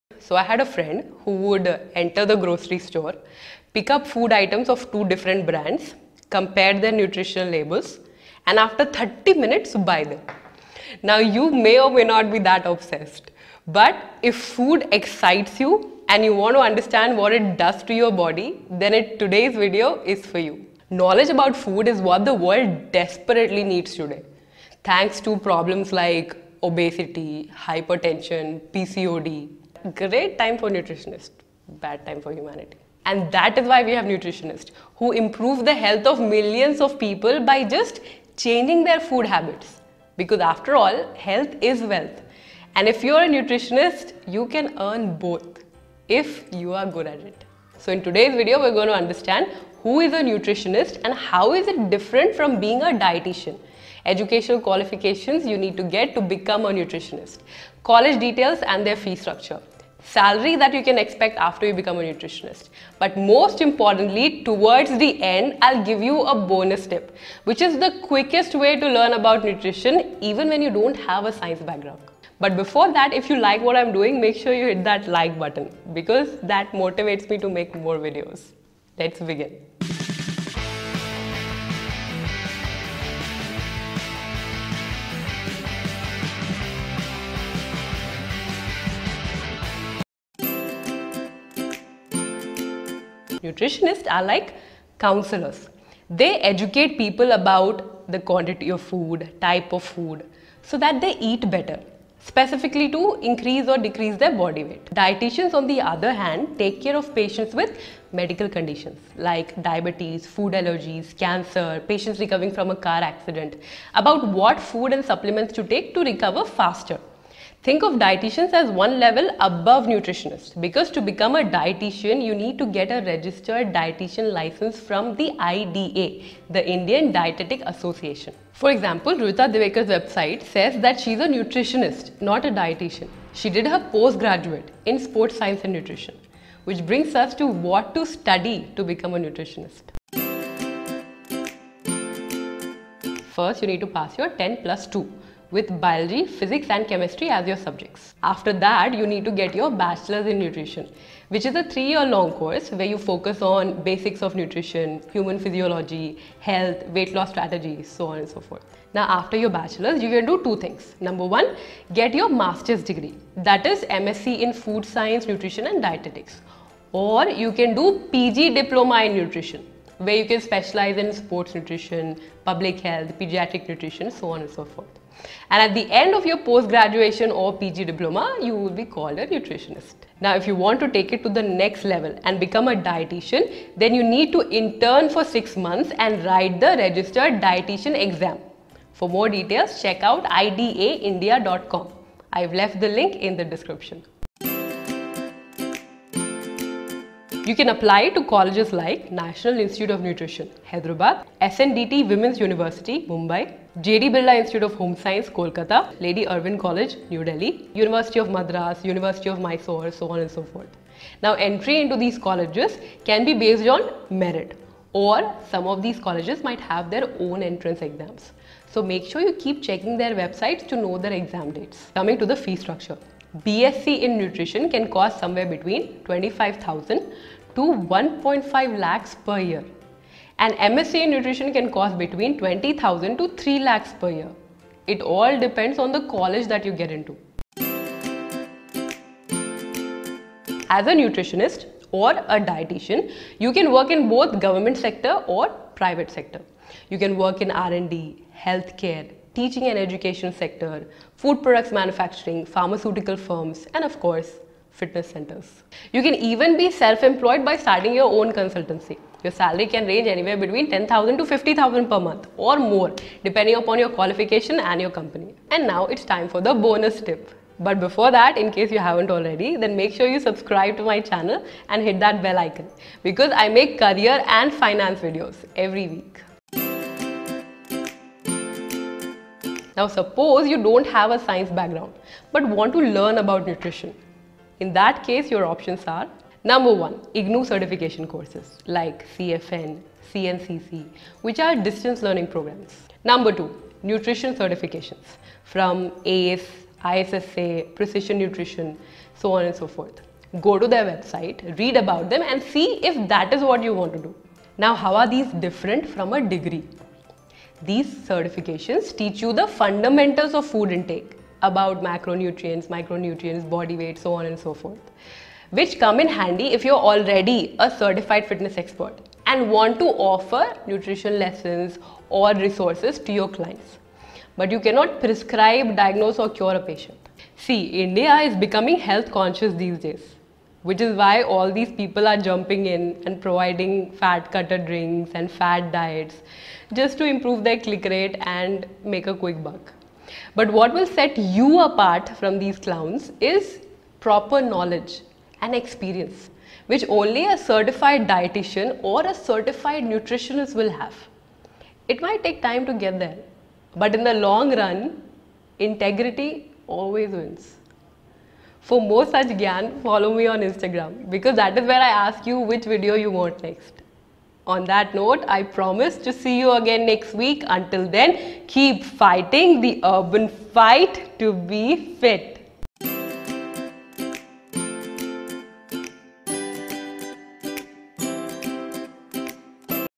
How to Become a Nutritionist in India? (Fees & Salary) | Registered Dietitian
ABOUT THE VIDEO:
So, I had a friend who would enter a grocery store, pick up food items of 2 different brands, compare their nutritional label and after 30 minutes buy them. Now you may or may not be that obsessed…but if food excites you and you want to understand what it does to your body, then today’s video is for you.

Knowledge about food, is what the world desperately needs today. Thanks to problems like obesity, hypertension, PCOD...

Good time for nutritionists, bad time for humanity.

And that’s why we have nutritionists who improve the health of millions of people by just changing their food habits because after all, health is wealth and if you are a nutritionist, you can earn both if you are good at it.

So in today’s video, we are going to understand:

1) Who is a Nutritionist & how is it different from being a Dietician?

2) Educational Qualifications you need to become a Nutritionist in India.

3) Colleges to apply to & their fee structure

4)  Salary you can expect after you become a Nutritionist (Salary and Career)

5)  But most importantly, towards the end, I’ll give you a Bonus Tip, which is the quickest way to learn about Nutrition even when you don’t have a science background.